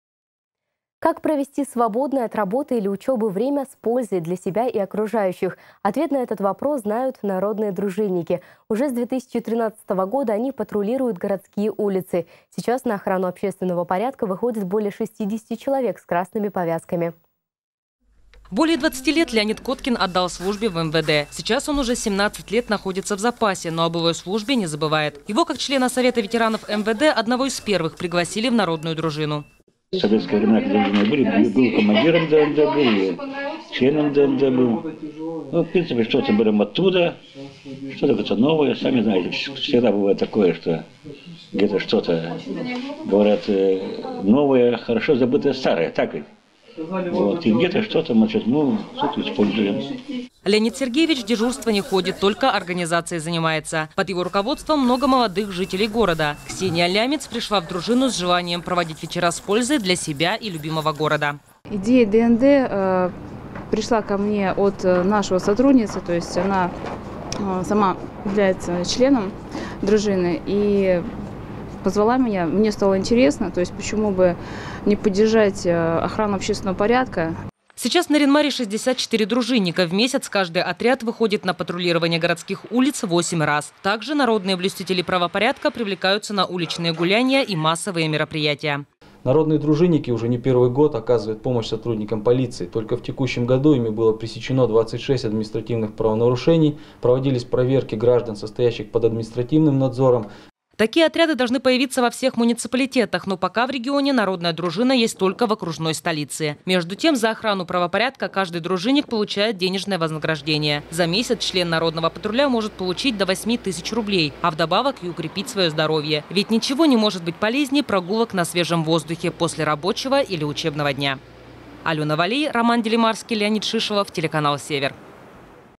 Народные дружинники Нарьян-Мара за год предотвратили 26 правонарушений
Как провести свободное от работы или учебы время с пользой для себя и окружающих? Ответ на этот вопрос знают народные дружинники Нарьян-Мара. Уже с 2013 года они патрулируют городские улицы. Сейчас на охрану общественного порядка по вечерам выходят более 60 человек с красными повязками.